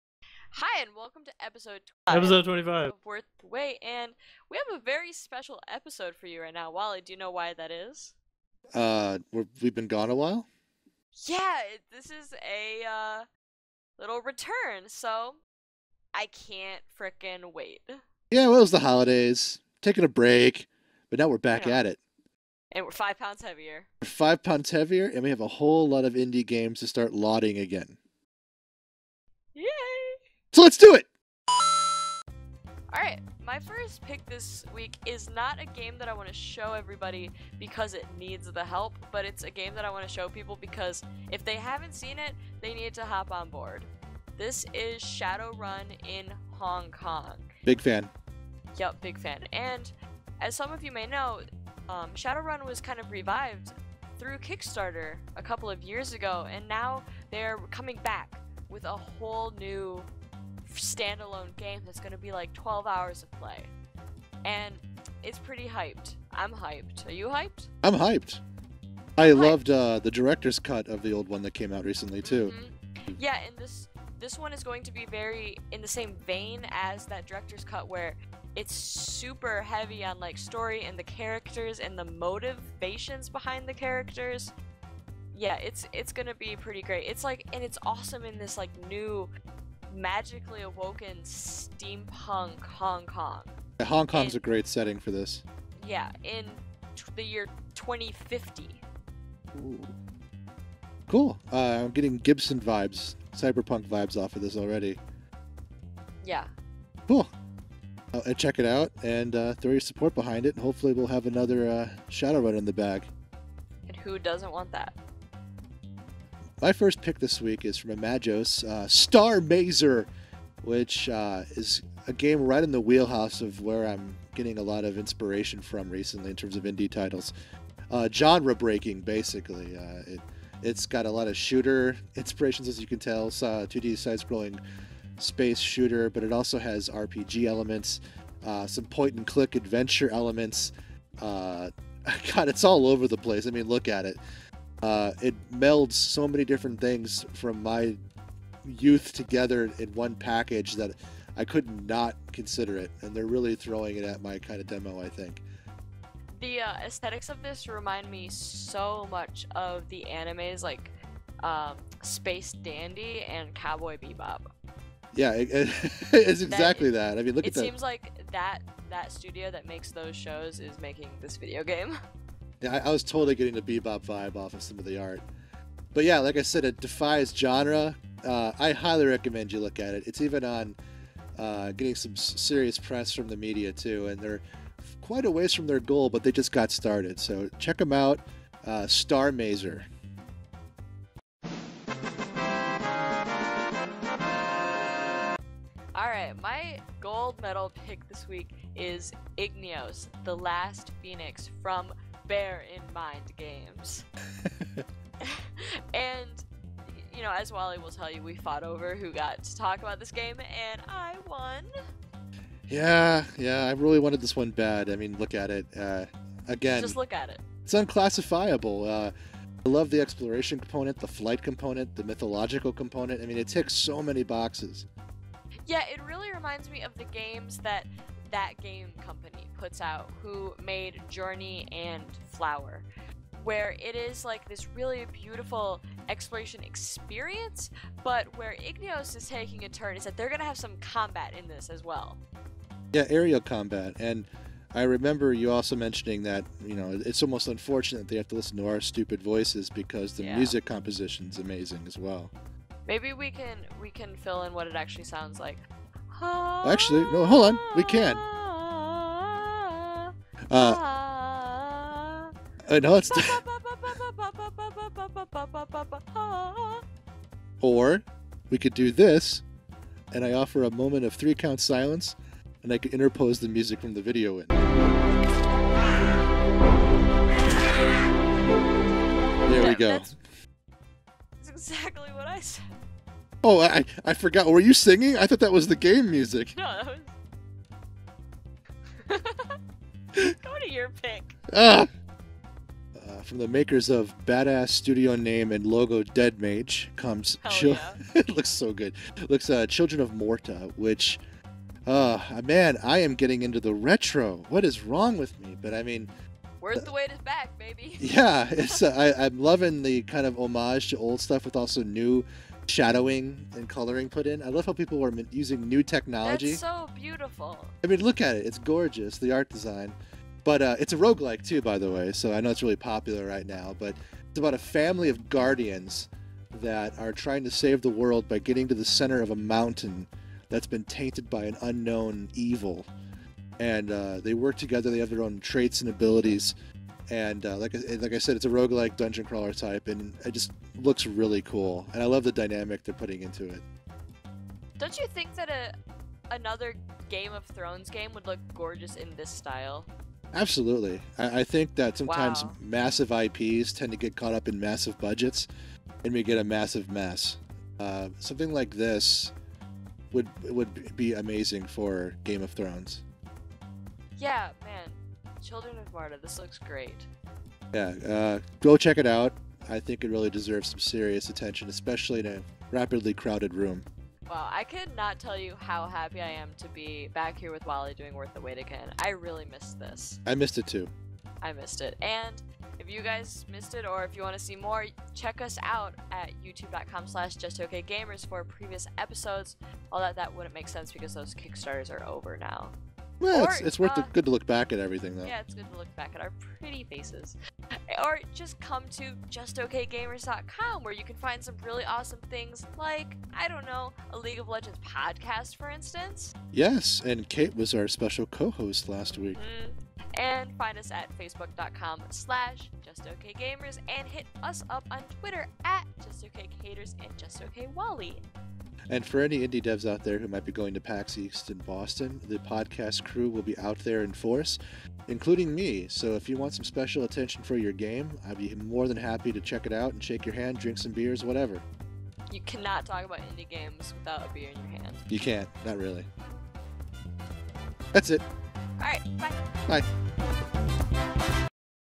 Worth the Wait - Episode 25
Welcome to Episode TWENTY-FIVE of Worth the Wait - A Just OK Gamers production where publicly funded games are highlighted for their merit.

Part of the Just OK Gamers family.